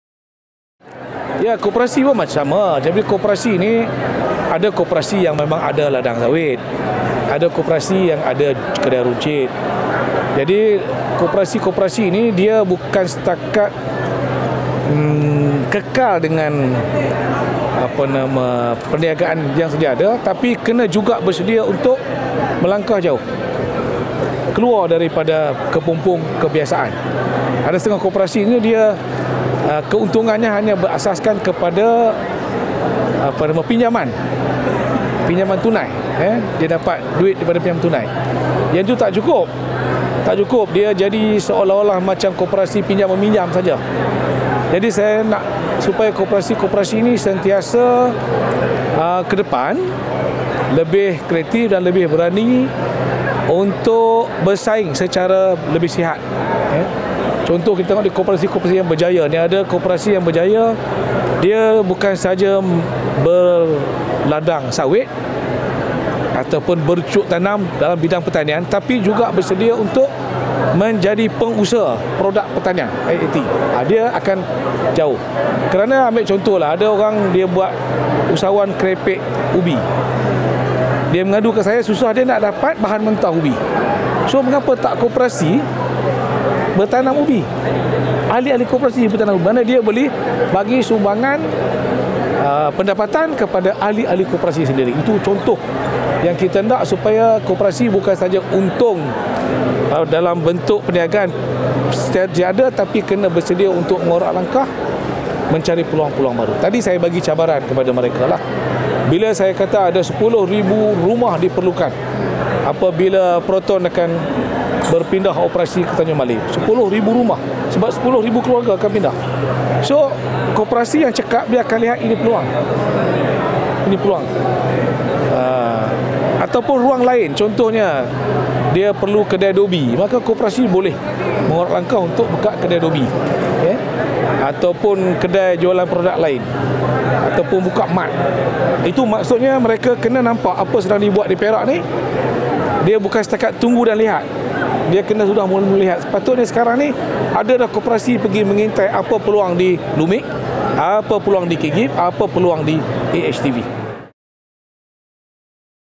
MGPerak : Perak Saran Koperasi Teroka Peluang Baharu, Ambil Manfaat Perkembangan AI
Semua koperasi di Negeri Perak disaran untuk meneroka peluang perniagaan dan perkhidmatan baharu termasuk mengambil manfaat daripada penggunaan kecerdasan buatan (AI).

Menteri Besar Perak, Datuk Seri Saarani Mohamad berkata, organisasi koperasi seharusnya tidak ketinggalan dalam arus perkembangan teknologi masa kini. - MGPerak